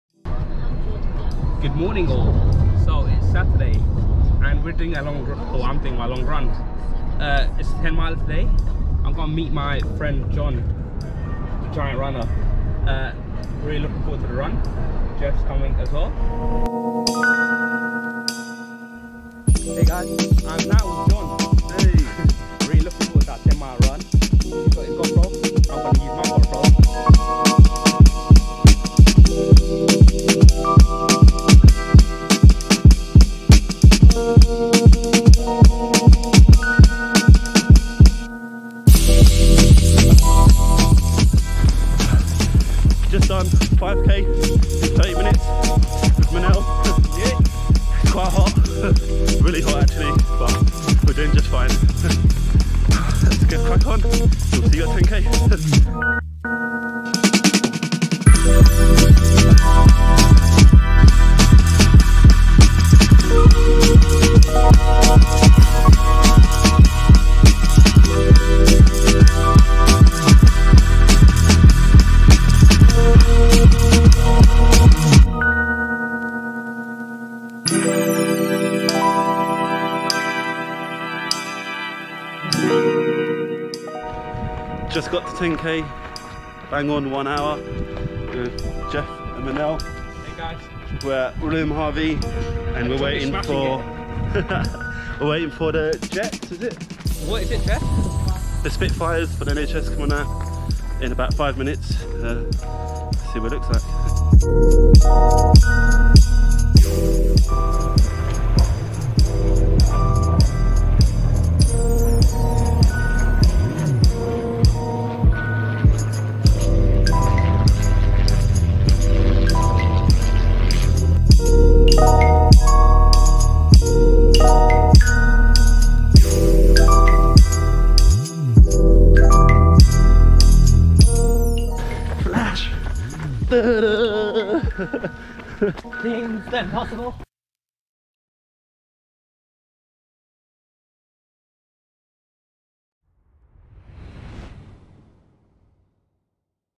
Long run Saturday in Ashford
10 miles in the bag for John and myself, and 10km for Geoff